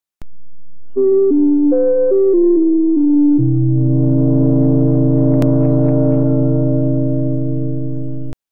Requested by Chaz (Pimenova Fan): Radio-Quebec logo (1969-1980s) remake
This took 8 minutes.

Done in Blender. SVG created by me. Nothing else to say. Yeah, the logo is that boring, but not scary.

Credit to Tele-Quebec